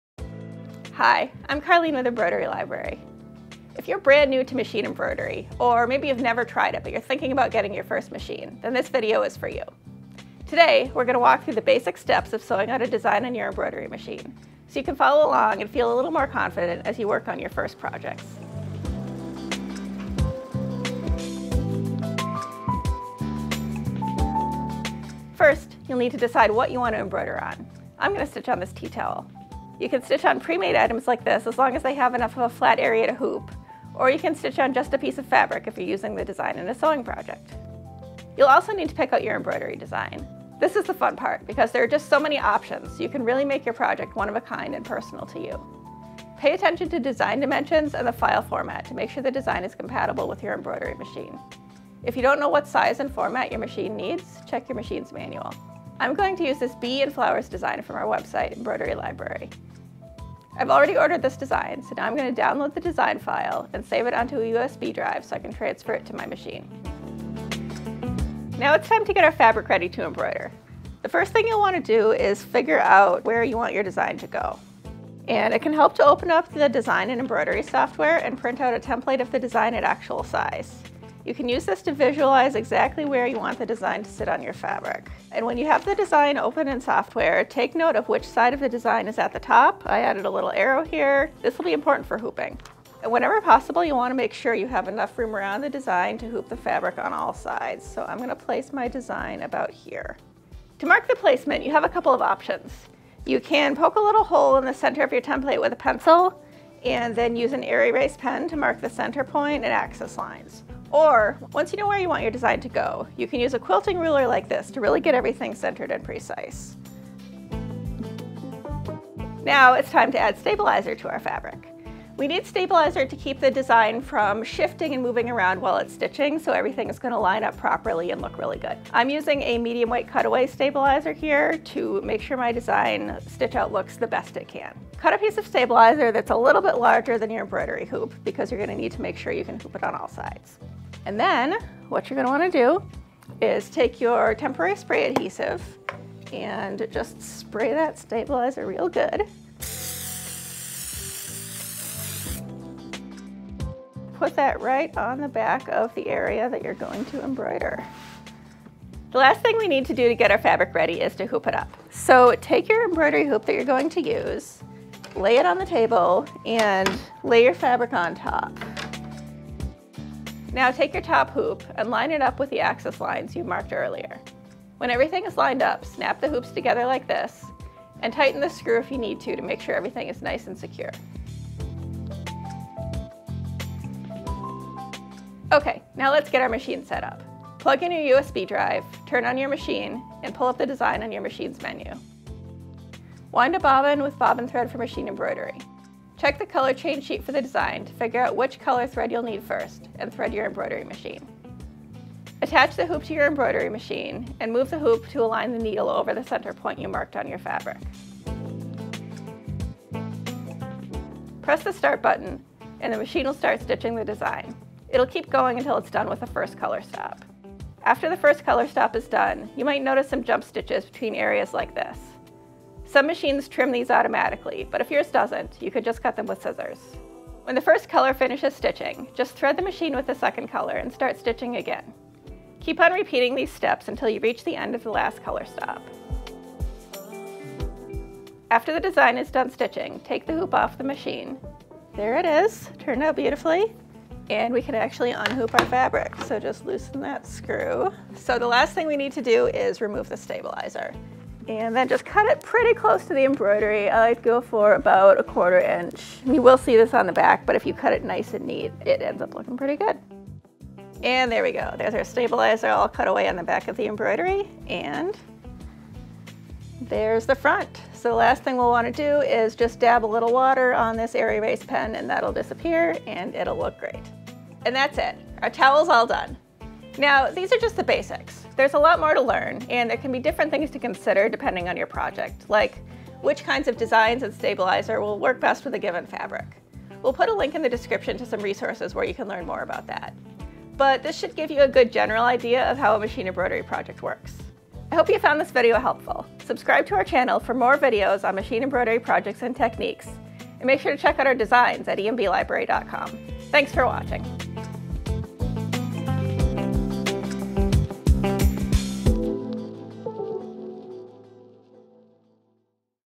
Your First Machine Embroidery Project
If you're a machine embroidery beginner or just curious about how to use an embroidery machine, this video is for you! We'll walk you through your first machine embroidery project, including choosing designs and fabric, stabilizing and hooping, stitching out your design, and finishing your project.

TIMESTAMPS
0:00 Introduction
0:24 Choose Your Fabric and Design
1:07 Prepare and Hoop Your Fabric
3:02 Set Up Your Machine
3:27 Stitch Your Design
3:58 Remove Excess Stabilizer
4:34 Conclusion